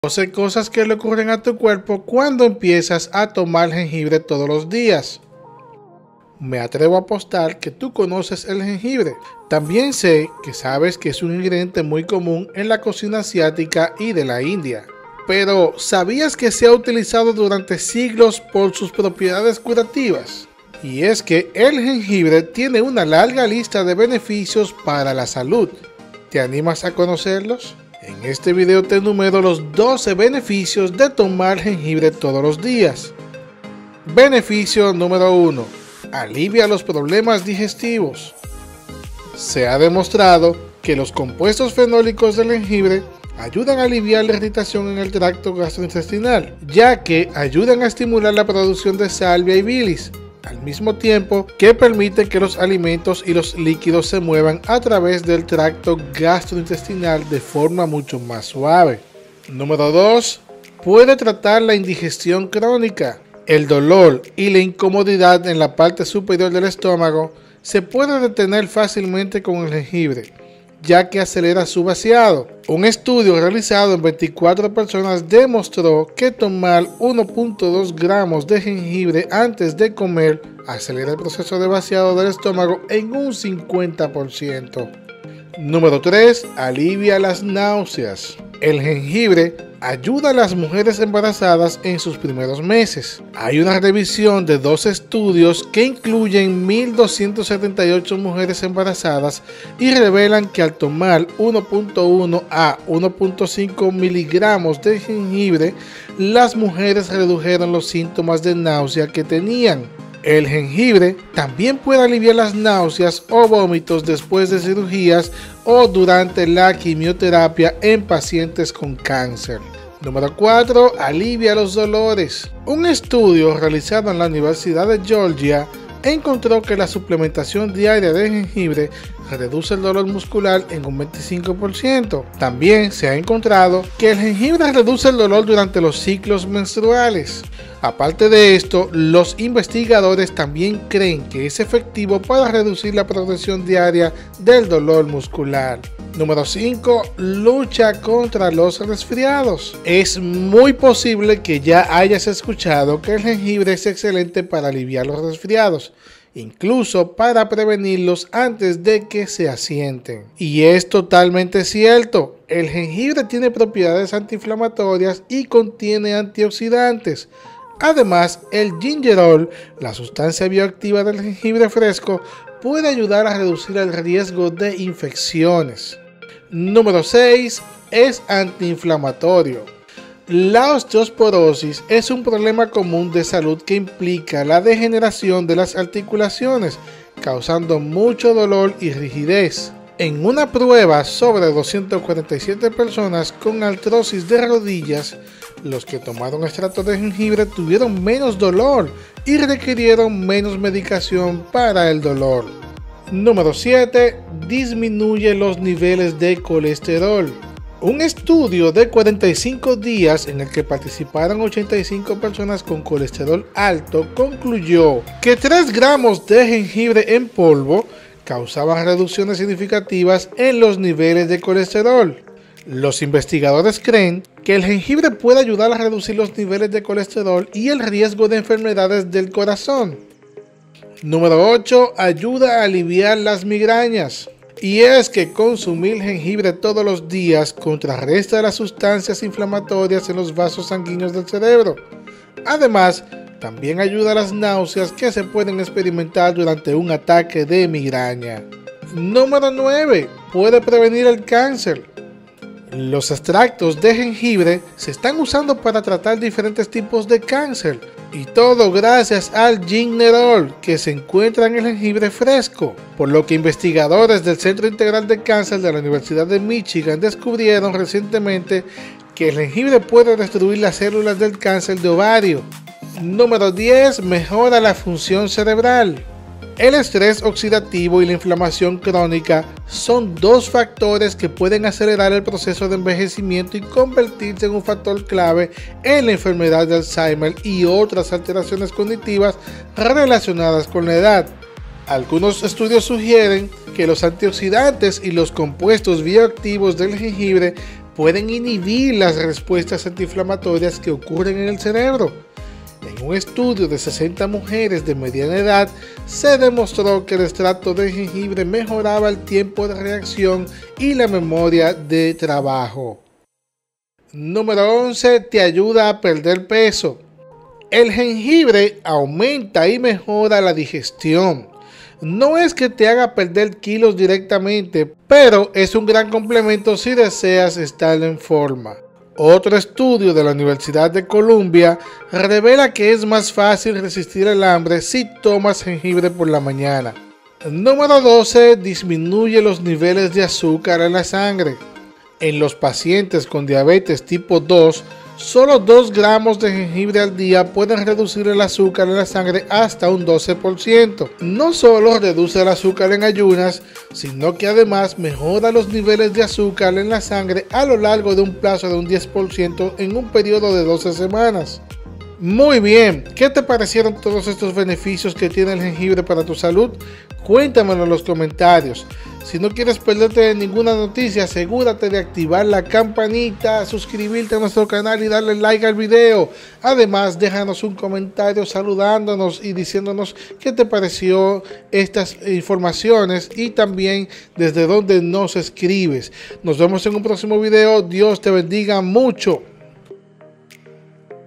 Nunca Nadie Jamas te ha dicho esto sobre el Jengibre
En este nuevo video te presentamos 12 beneficios de consumir jengibre todos los dias, y como eso aporta a tu salud.